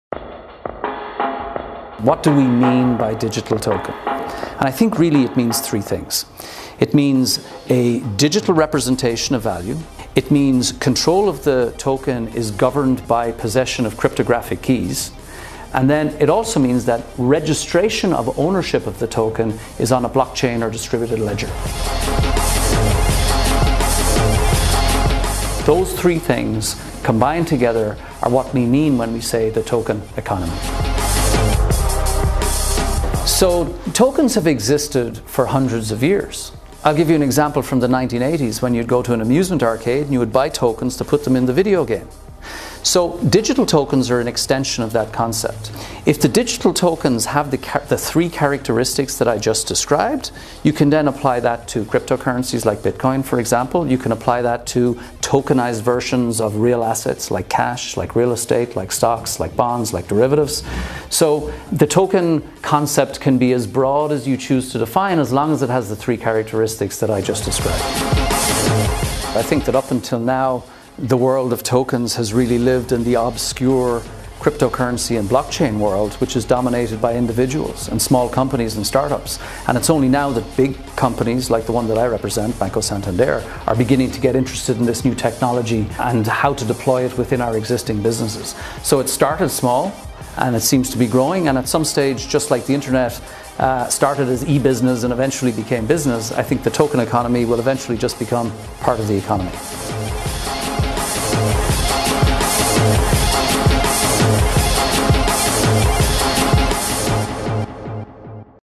¿Qué son exactamente los tokens digitales?
"Los tokens existen en realidad desde hace cientos de años. Por ejemplo, en 1980 cuando ibas a las salas de recreativos,
comprabas tokens  y las insertabas en los
videojuegos. Los tokens digitales son en realidad una extensión de
ese concepto." Hablamos con John Whelan, responsable de Banca de Inversión Digital de Banco Santander, en el marco del Evento Retina Blockchain y la economía del token realizado en Utopicus el pasado 22 de enero.